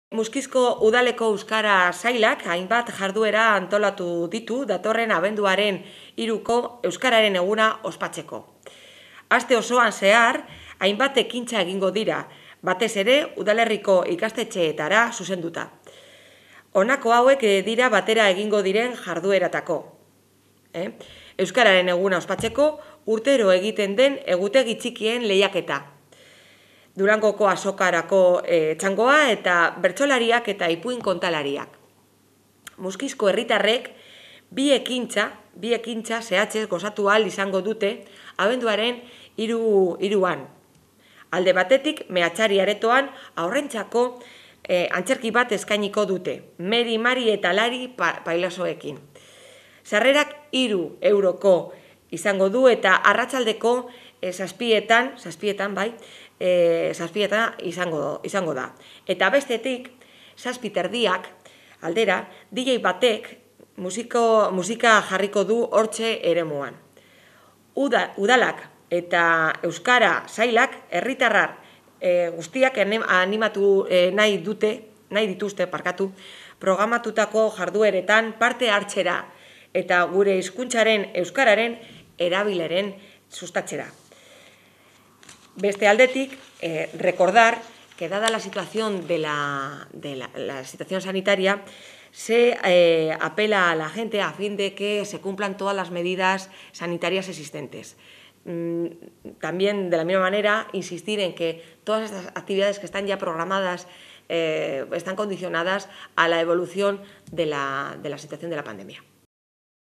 Música y espectáculo infantil para celebrar el Día del Euskera en Muskiz
El Ayuntamiento ha programado, para mañana por la tarde, una sesión con los payasos Meri, Mari eta Lary, en Meatzari Aretoa.

La jornada festiva continuará con música a cargo de un DJ en el Ortze.